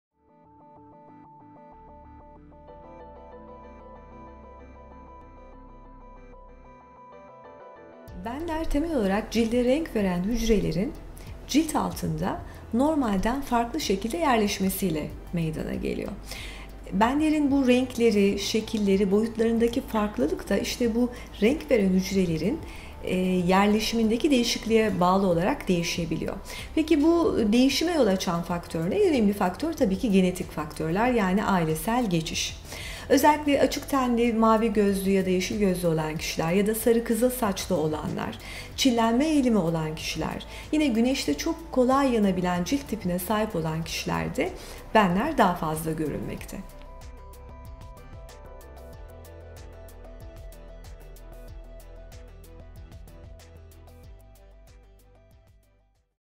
Cilt Benleri Neden Oluşur - Prof. Dr. Gonca Gökdemir
Prof. Dr. Gonca Gökdemir "Cilt Benleri Neden Oluşur?" sorusunun cevabını veriyor.